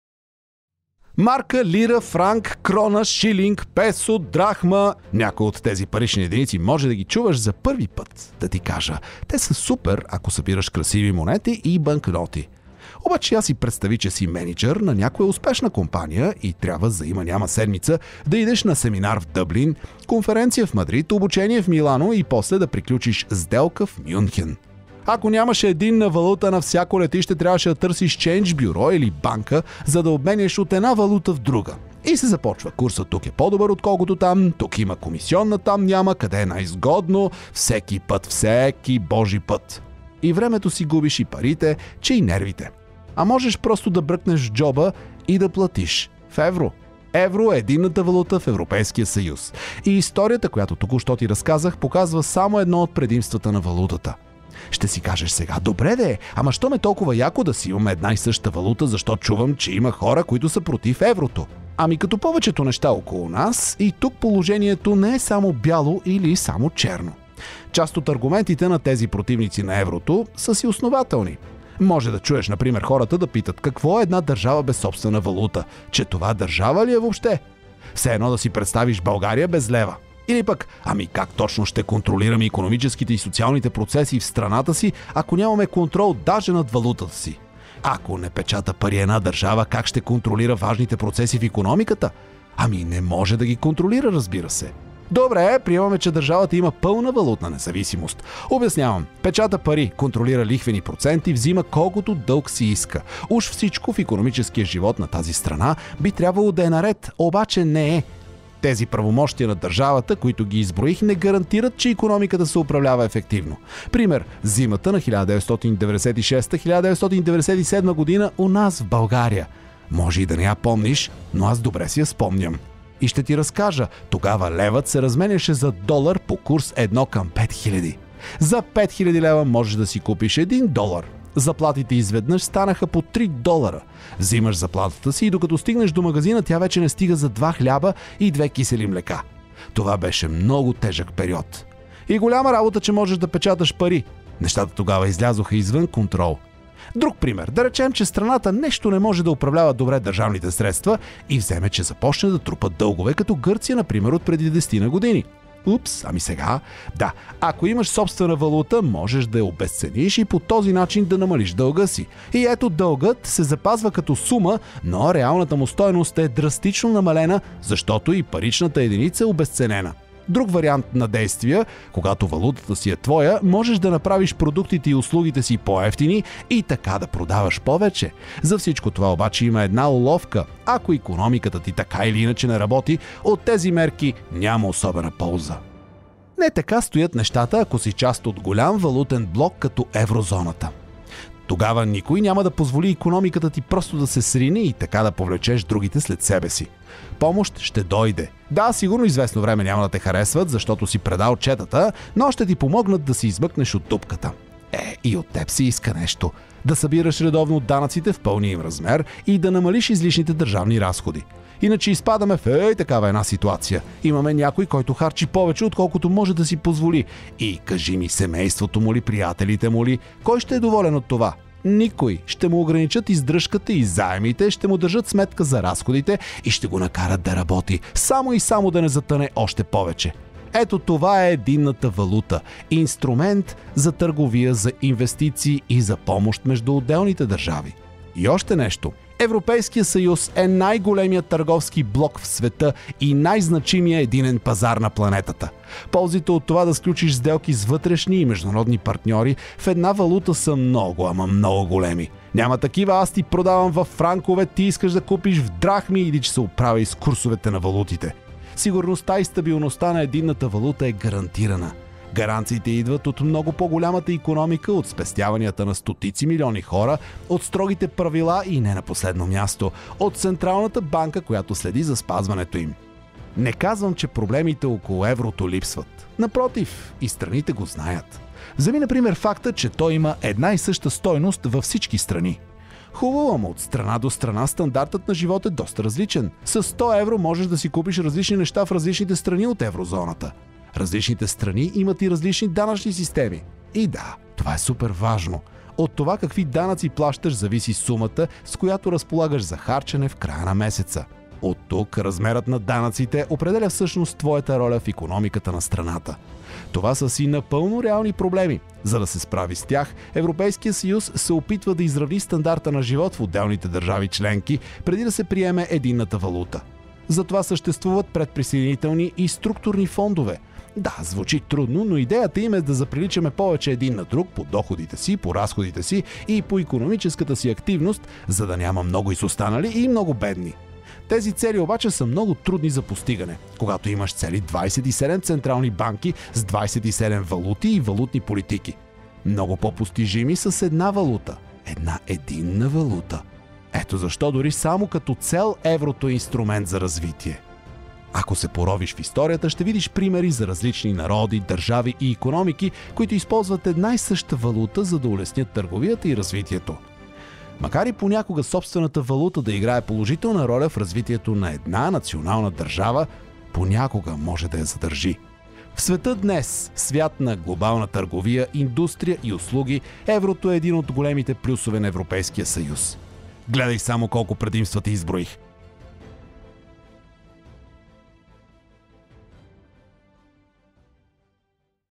За членството в еврозоната
„Дори само като цел, Еврото е инструмент за развитие.“
Ако искаш да разбереш повече, гледай последното видео от образователната ни поредица, посветена на ЕС. Тя е създадена от фондация „Софийска платформа“ в партньорство с образователния сайт @Уча.се и с подкрепата на посолството на Германия в София.

"The euro is an instrument for development and something to aspire to".
If you want to learn more, watch the last video of our educational series, dedicated to the EU.